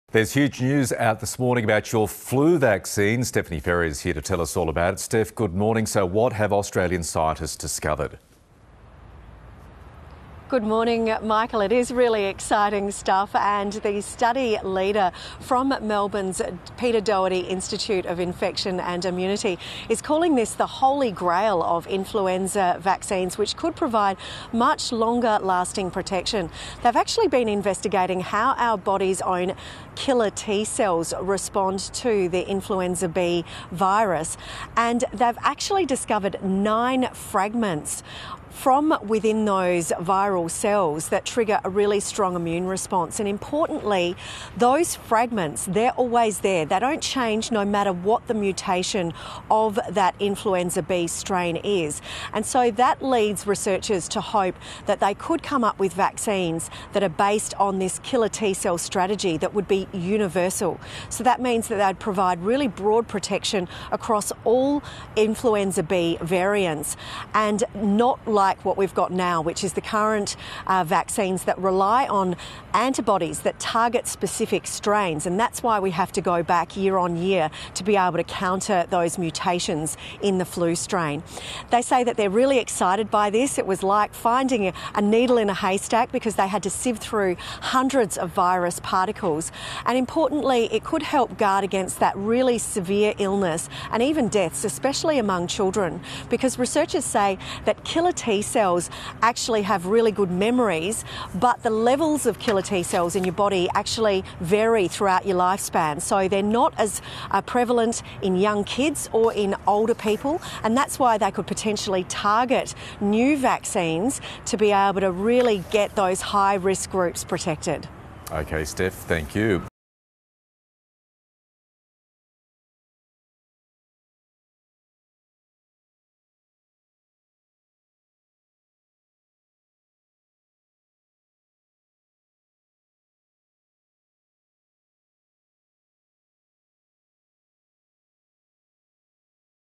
Universal flu vaccine breakthrough | ABC News
Medical scientists are getting closer to developing a universal flu vaccine, which could work on future mutations of the virus.
Research from the Doherty Institute has identified new fragments of the influenza B virus that our immune system can attack, and could be used in the development of a long lasting vaccine.